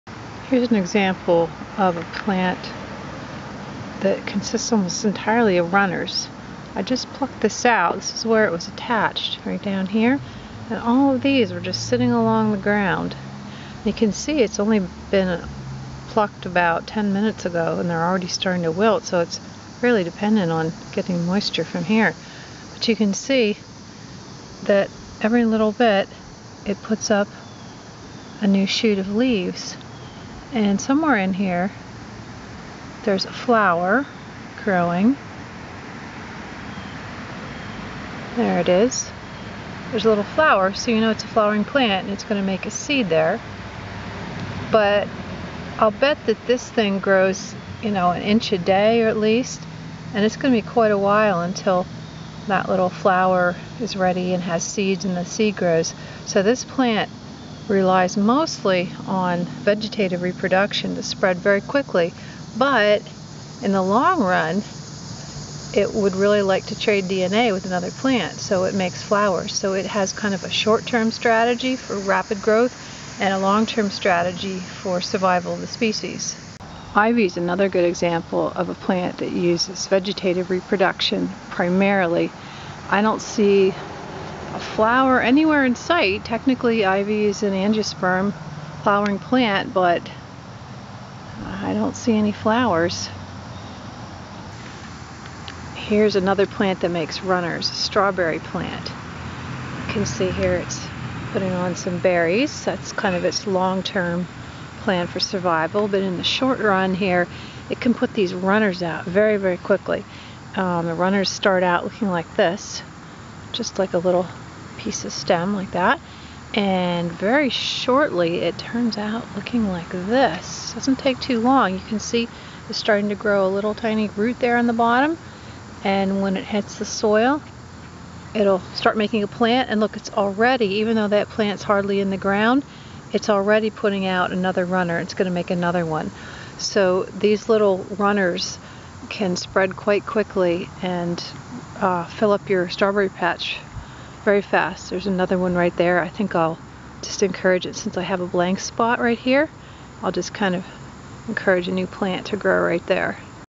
Plants with runners
Some samples of plants that rely on runners as primary form of reproduction.